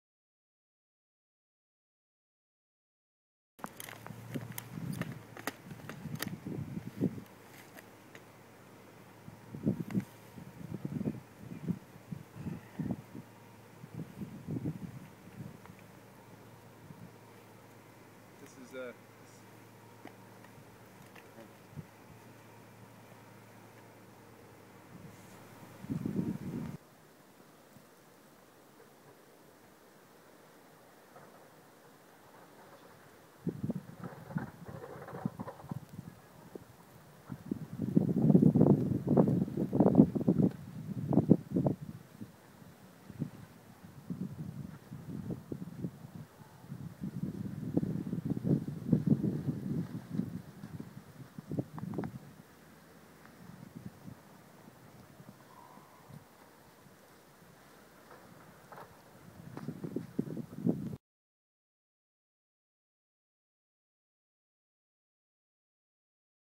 Jan312012 SealCoveStandOff kscs.wmv
RNC respond to reports that an individual has barricaded himself somewhere on a property in Seal Cove on January 31st 2012